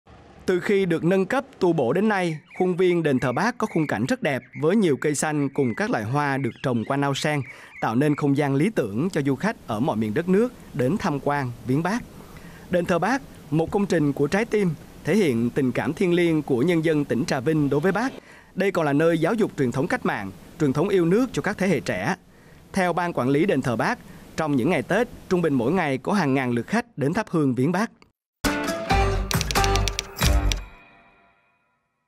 HÀNG NGÀN LƯỢT KHÁCH ĐẾN VIẾNG ĐỀN THỜ BÁC TRONG DỊP TẾT | VTV5
Khuôn viên Đền thờ Bác có khung cảnh rất đẹp, với nhiều cây xanh cùng các loại hoa được trồng quanh ao sen, tạo nên không gian lý tưởng cho du khách ở mọi miền đất nước đến tham quan, viếng Bác. “Đền thờ Bác – một “Công trình của trái tim”, thể hiện tình cảm thiêng liêng của Nhân dân tỉnh Trà Vinh đối với Bác.